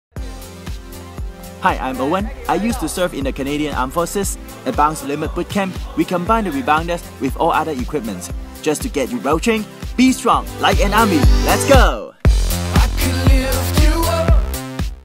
Program: Bootcamp
This type of targeted training is for those who want to see faster results, the workout will target the core, butt, and legs to get you that bikini body. We'll be using a mix of training styles and  equipment to rev up your training and optimise your results. The training sessions will vary between different styles of training such as TRX, body weight and the rebounder.